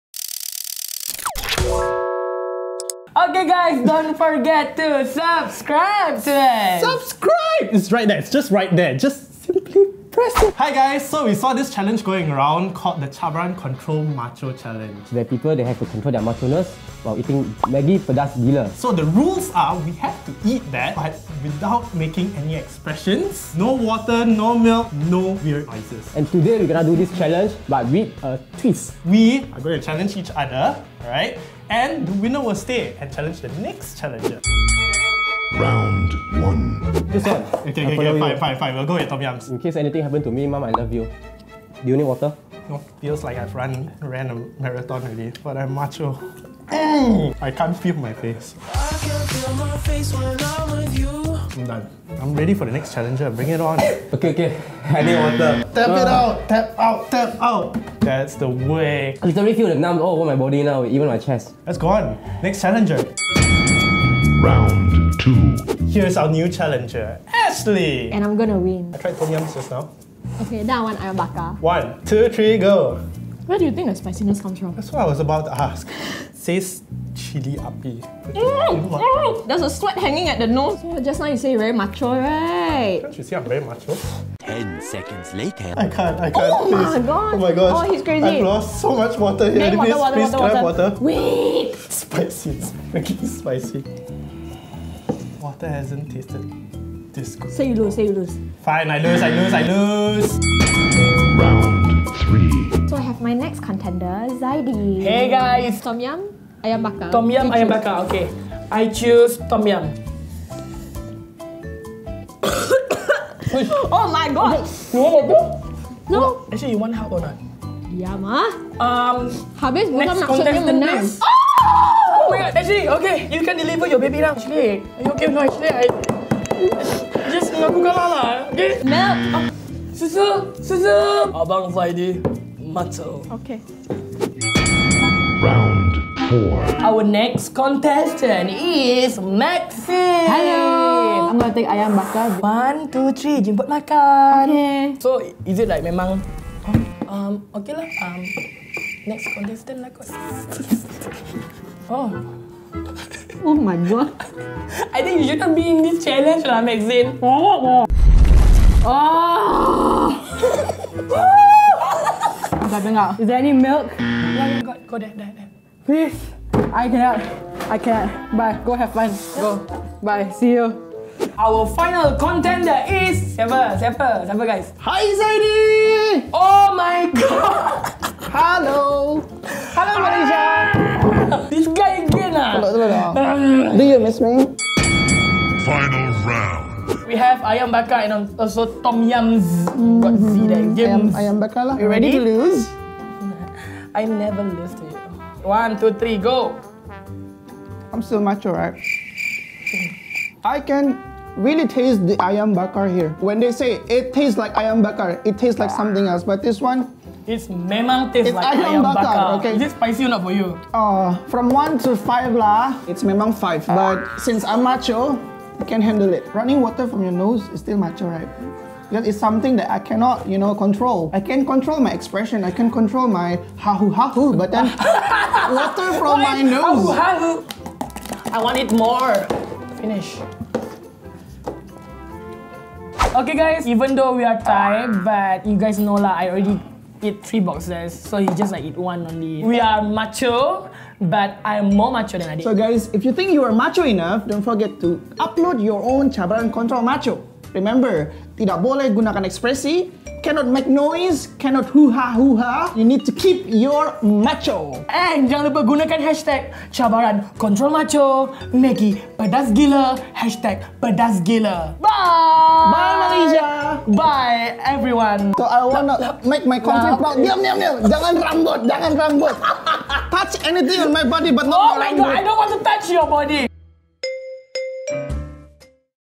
We Do The CabaranKontrolMacho | Presented by Maggi Pedas Giler
We took on the cabarankontrolmacho and we ate Maggi Pedas Giler Instant Noodles. It was a touch challenge. Watch how much our colleagues suffer. Do you dare to take the challenge?

Talents Featured:
Ashley

Production Crew:
Producer - Kenneth Leow
Videographers - Amirul Rahman
Audio - Nicholas Tan
Editor - Maxine Goh

Next up: Maggi Pedas Giler 2X